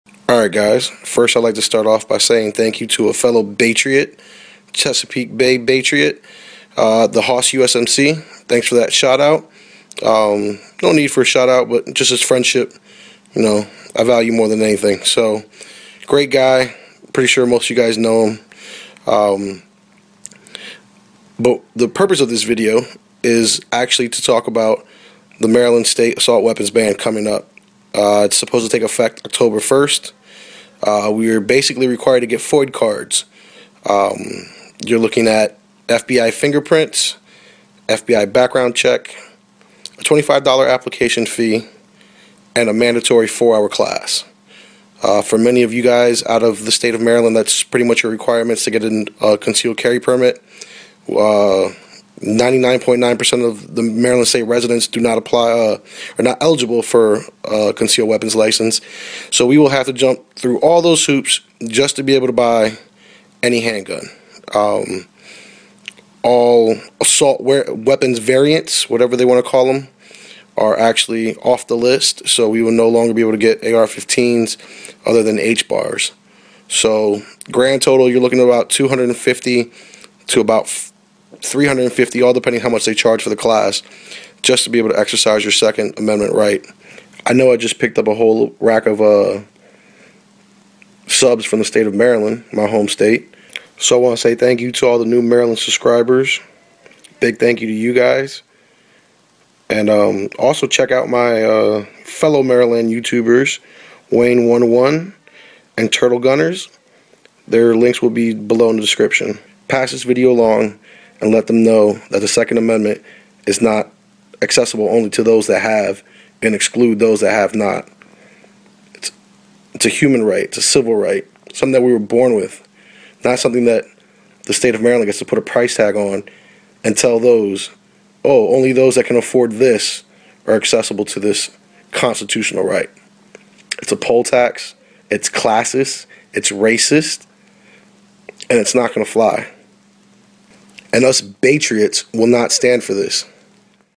To all Maryland Baytriots
even if you dont think people should have guns are you really OK with putting a price tag on a constitutional right? next it could be a right that you cherish that comes with a dollar amount. can you imagine being charged 200-300 dollars for, the right to free speech, speady trial, or the right to be protected from illegal search and seizure.